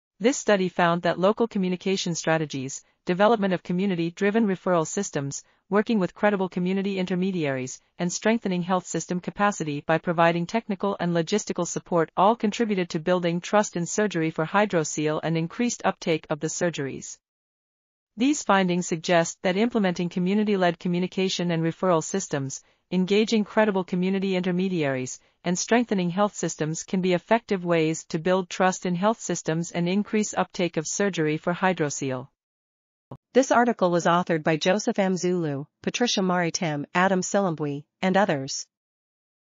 Unlocking Trust in Community Health Systems: Lessons From the Lymphatic Filariasis Mo... | RTCL.TV
### Keywords ###

### Article Attribution ###
Title: Unlocking Trust in Community Health Systems: Lessons From the Lymphatic Filariasis Morbidity Management and Disability Prevention Pilot Project in Luangwa District, Zambia
Authors: Joseph M. Zulu, Patricia Maritim, Adam Silumbwe, Hikabasa Halwiindi, Patricia Mubita, George Sichone, Chileshe H. Mpandamabula, Frank Shamilimo ,and Charles Michelo
Publisher: Kerman University of Medical Sciences
DOI: 10.34172/ijhpm.2021.133

### Video Timestamps ###
0:00:00 - Summary
0:00:33 - Title
0:00:39 - End